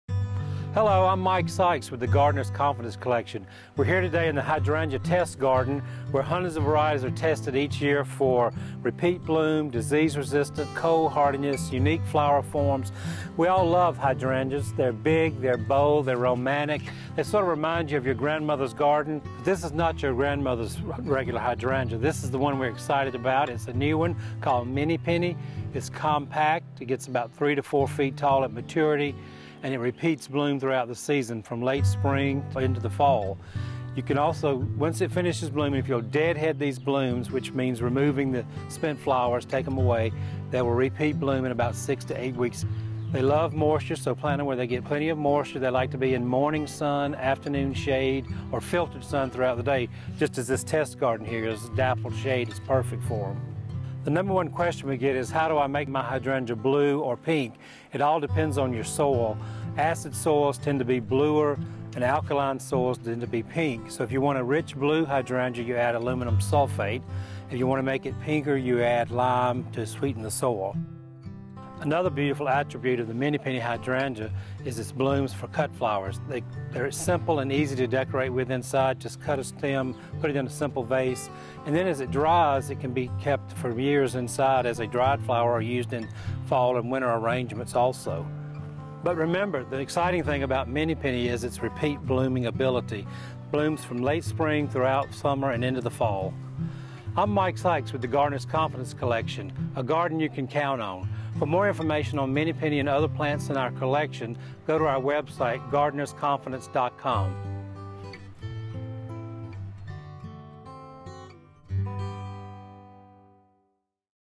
Reblooming Hydrangeas Offer More Bloom For Your Buck
Get more bloom for your buck with hydrangeas that show color summer through fall. For years, researchers have worked hard to find hydrangeas that bloom two times per year. The Mini Penny Hydrangea is a new, compact variety that is sure to be twice as rewarding. Master horticulturist, Mike Sikes shows off the blooms of this outstanding variety.